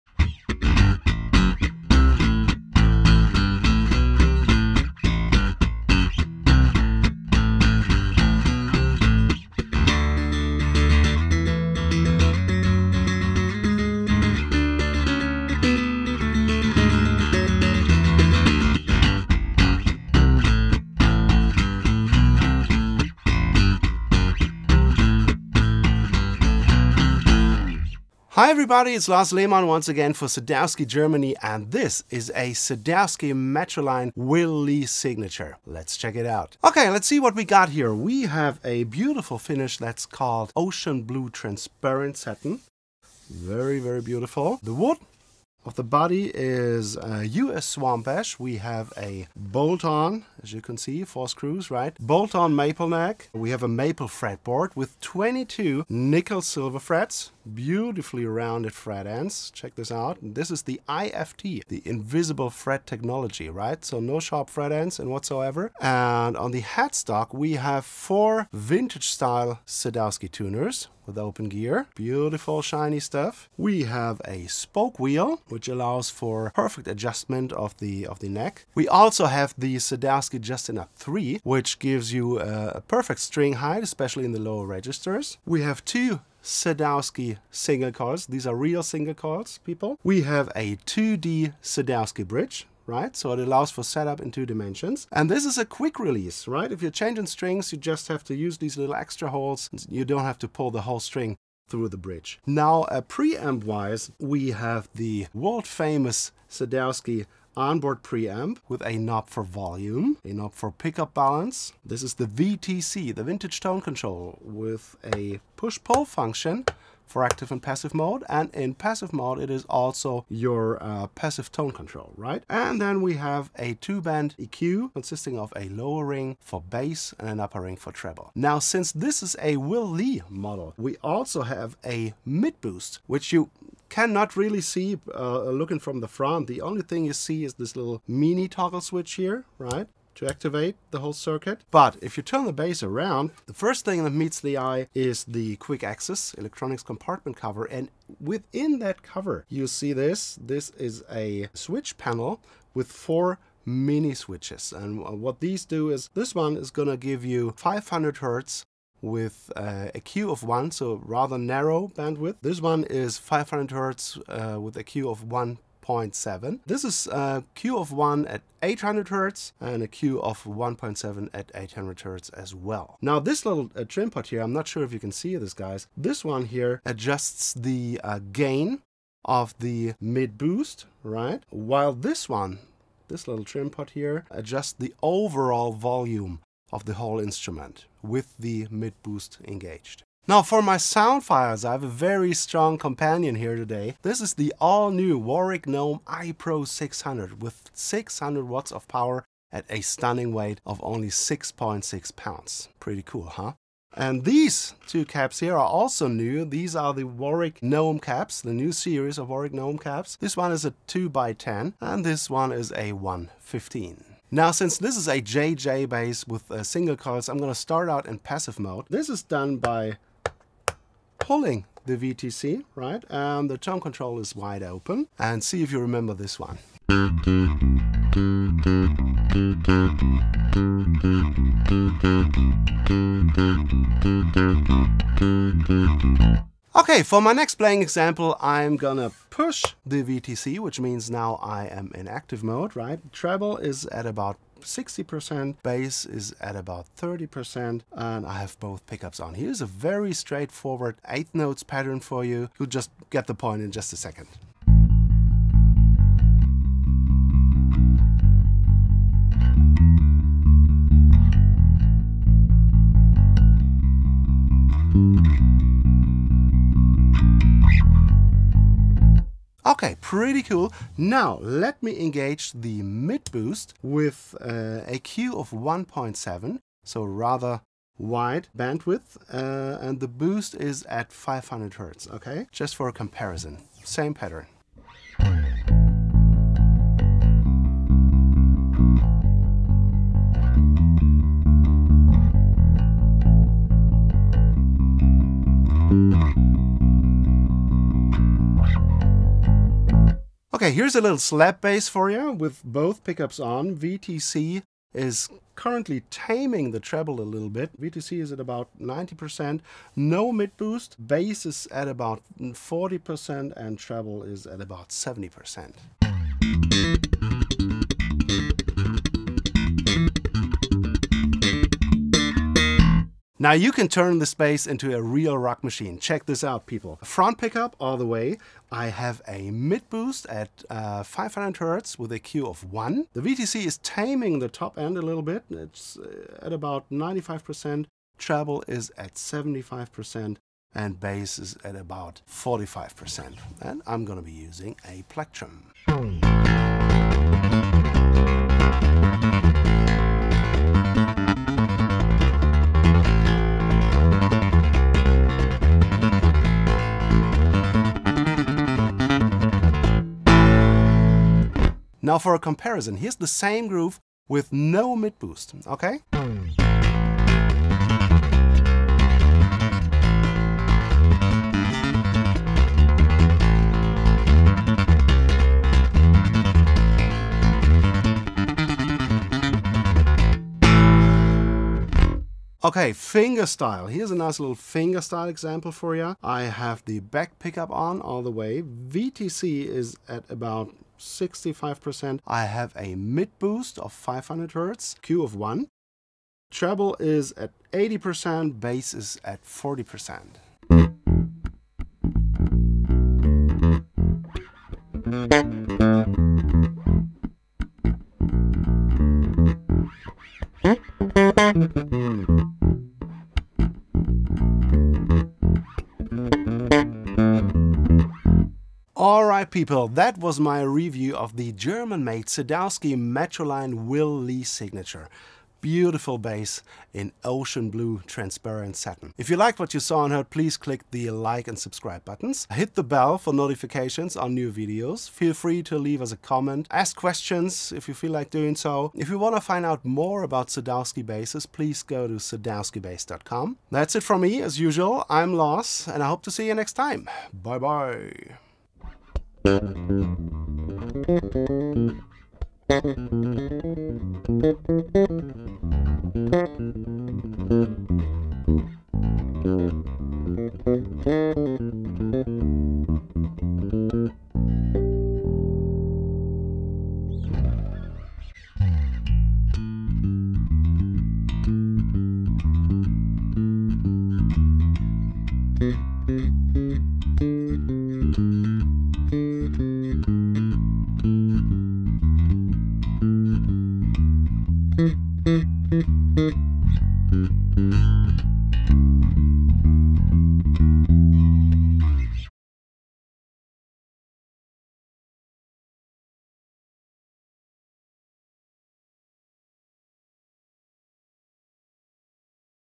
Mastering Bass Perfection: Sadowsky MetroLine Will Lee Signature 4-String Model | Made in Germany
Sadowsky MetroLine 22-Fret Will Lee Artist Line, Swamp Ash Body, 4-String - Ocean Blue Transparent Satin Finish | Article Code: SML22WL4 02 ASH FR

Discover the Sadowsky MetroLine Will Lee Signature bass in Ocean Blue Transparent Satin, presented by Lars Lehmann for Sadowsky Germany. This bass is a testament to precision craftsmanship, featuring an American Swamp Ash body, a bolt-on Maple neck with a Maple fretboard hosting 22 nickel silver frets. With a J/J pickup configuration, true bypass active electronics, and a custom Will Lee preamp, this bass delivers the iconic Sadowsky tone – a perfect blend of powerful lows, balanced mids, and sparkling highs. The innovative mid-boost feature adds punch at 500 Hz and 800 Hz. With Vintage Tone Control, treble and bass boost, and Sadowsky's Quick Release Bridge, this bass offers exceptional playability and control. Embrace the future of bass with the MetroLine Will Lee Signature bass – where modern sound meets classic design. Visit "sadowskybass.com" for more information.

Specs:
Sadowsky MetroLine 22-Fret Will Lee Artist Line Bass
Right-hand version
4-string electric bass
34" (864 mm) long scale
Bolt-on construction
American Swamp Ash body
Maple neck
Maple fingerboard
12" fingerboard radius
Sadowsky Just a Nut III
1.45" (36.8 mm) nut width
22 nickel silver frets (width: 0.094" (2.4 mm) / height: 0.051" (1.3 mm))
Passive Sadowsky J-Style pickup (neck)
Passive Sadowsky J-Style pickup in 60s position (bridge)
Custom Will Lee Peamp (on/off mid-boost, choice of 500 Hz or 800 Hz, wide or narrow bandwidth)
Controls for Volume / Balance / Vintage Tone Control (P/P) / Treble and Bass (stacked)
midboost switch
Push/pull function at VTC knob for preamp bypass
Sadowsky Light machine heads with open gear
Sadowsky S-security locks
Sadowsky bridge with quick string release
Chrome hardware
Sadowsky Blue Label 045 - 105 strings
Blue Transparent Satin finish
Parchment pickguard
Weight ca. 8.9 lbs (4.0 kg)
Incl. Sadowsky Fretboard Protector
Incl. hex key tools and individual Certificate of Authenticity
Incl. Sadowsky Portabag (SAC BAG BASS PORTA)

Or check your local dealer for availability.

00:00 - Intro Jam
00:28 - Introduction
00:36 - Specs
01:42 - Sadowsky - Will Lee Preamp
03:10 - Sound Examples
06:58 - Outro
07:28 - More Sadowsky Products